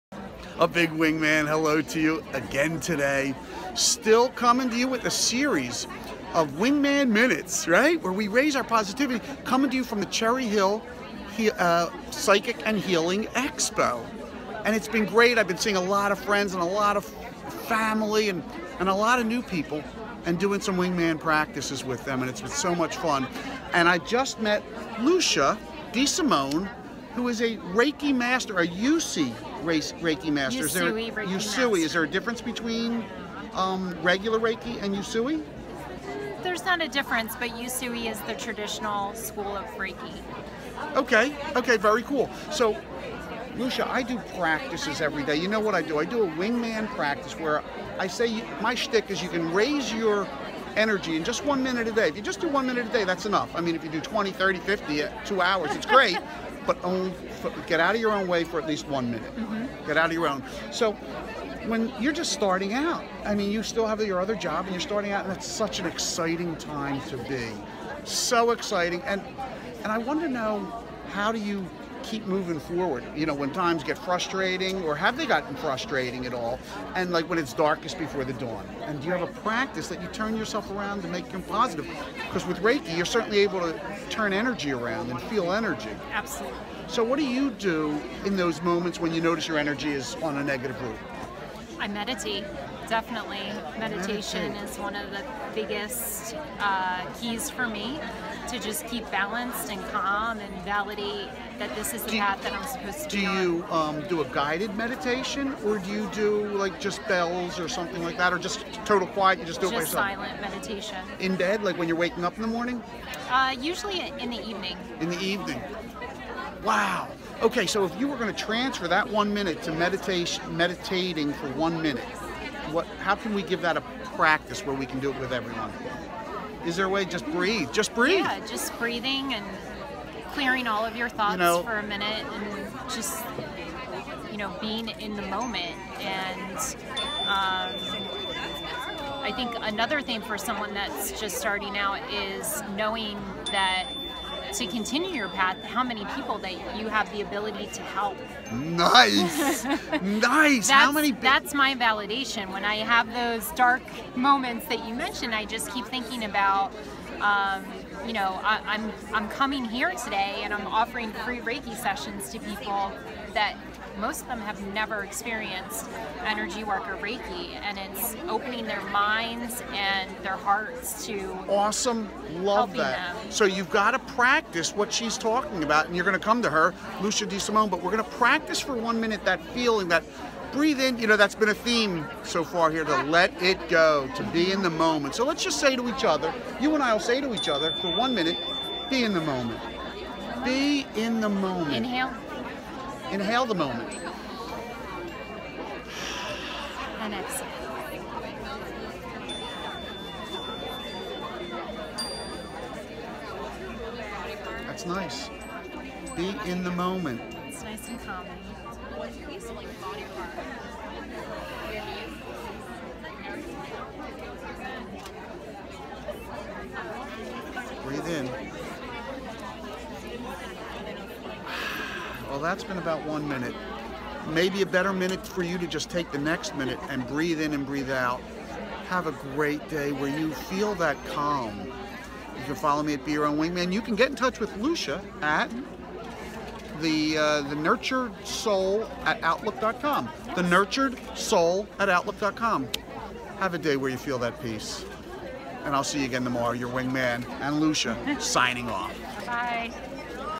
Breathe in, breathe out, with Lucia Disimone.
Lucia Disimone joins Michael for a wingman minute of peace.